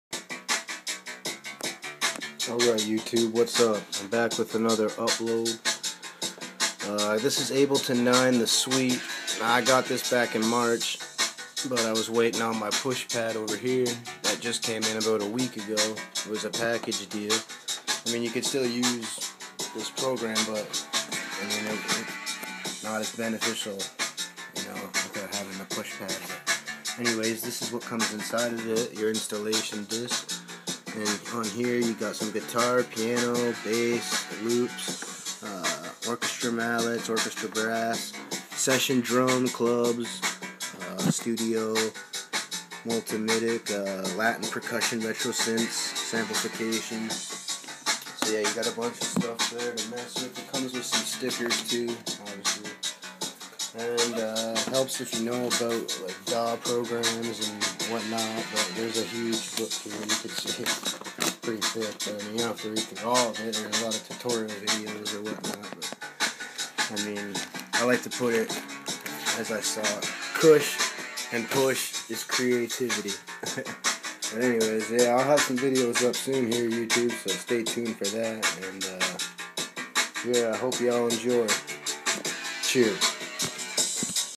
Ableton Live 9 Suite Unboxing Intro
Intro video of what you get with Ableton Live 9 Suite, with free upgrade dl.

Push as well.